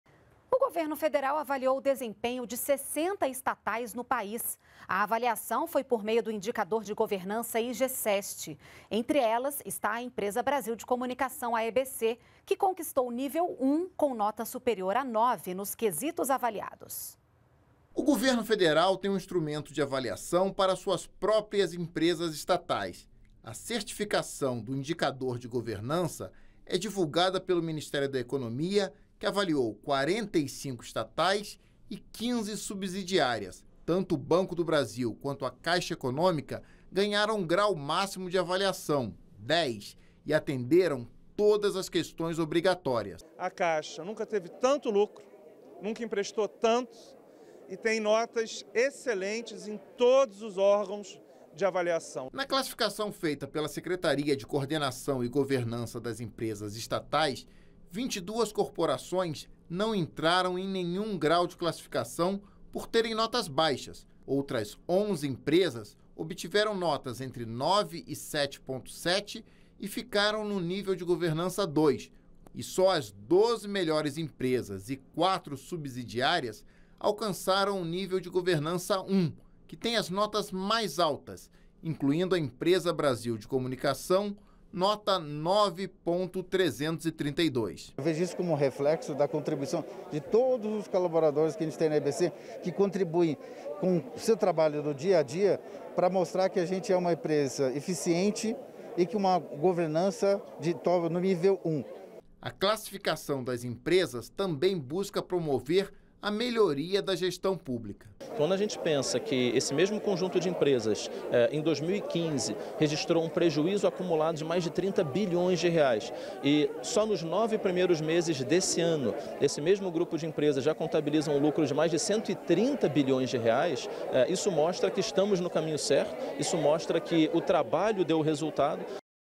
EBC conquista nível 1 em avaliação do governo federal sobre estatais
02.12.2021 - O governo federal avaliou o desempenho de 60 estatais no país por meio do Indicador de governança IG-SEST. Entre elas, está a Empresa Brasil de Comunicação. A EBC conquistou nível 1, com nota superior a 9 nos quesitos avaliados.